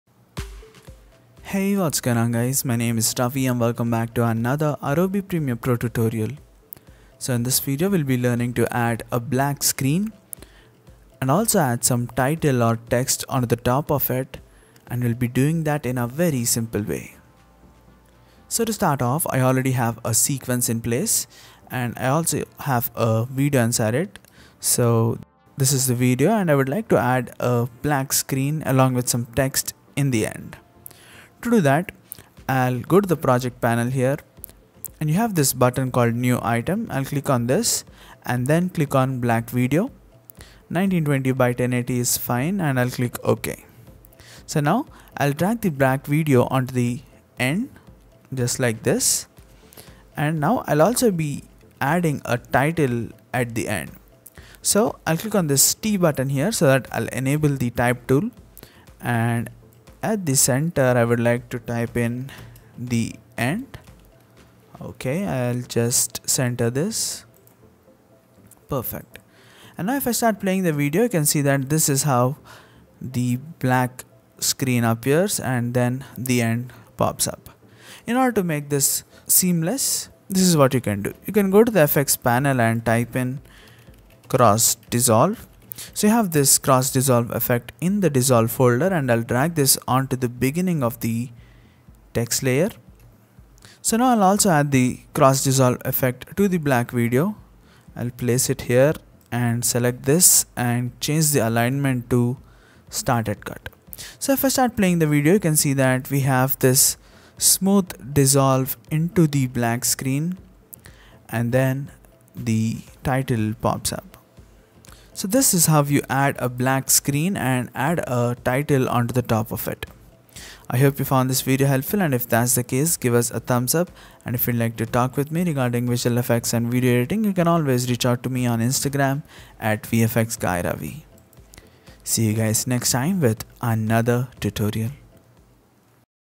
How to Add Black Screen with Title or Text | Adobe Premiere Pro Tutorial
Learn How To add black screen along with text or using Adobe Premiere pro. We will be creating a new item called Black video and will be adding it to our time line. We will also be using the type tool to create a title or text. In addition to that, we will also use the effect called cross dissolve to add a smooth transition

➔ Stay Connect With iraFX Studio